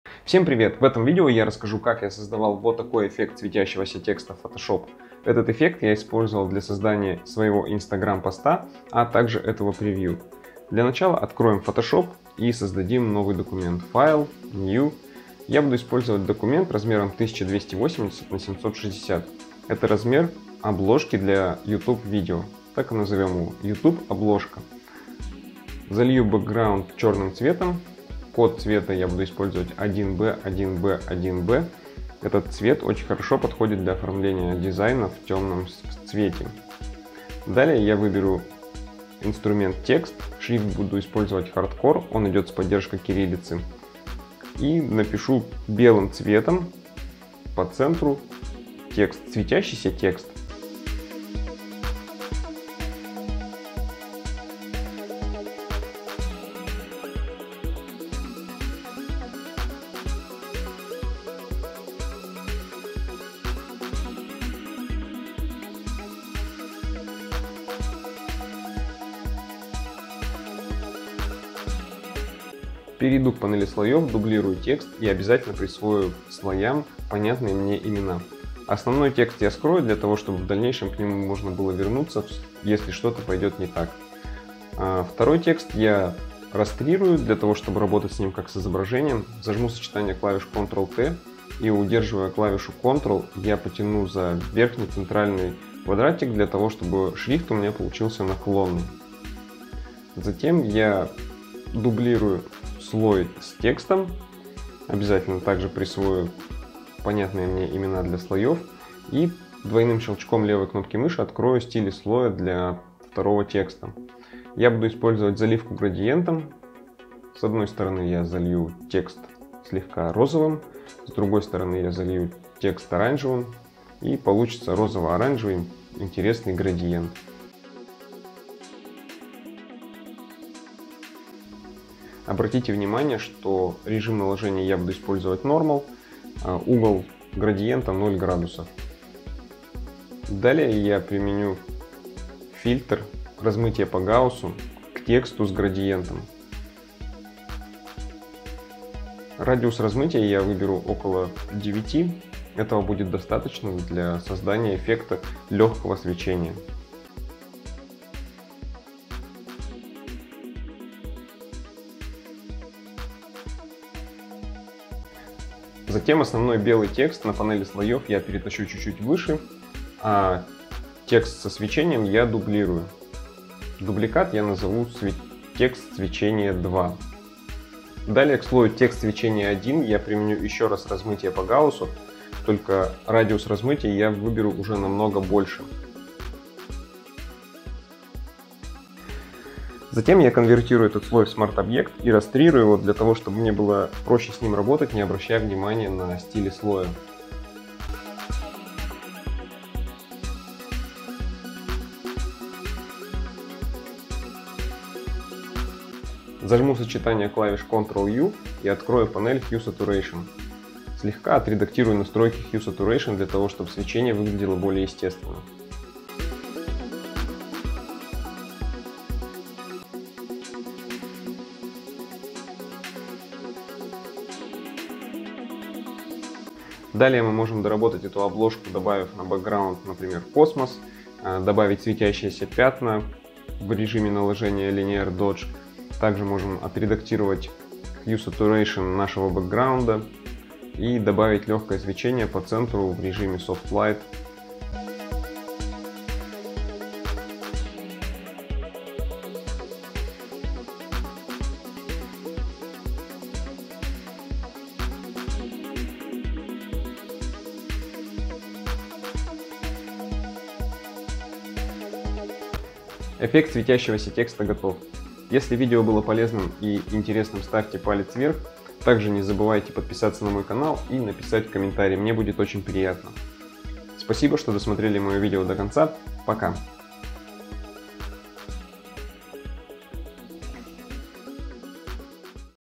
Как сделать светящийся текст в Фотошопе. Простые уроки Photoshop для новичков. Свечение текста
Видео о создании светящегося текста в Фотошоп.
Полное описание добавлю позже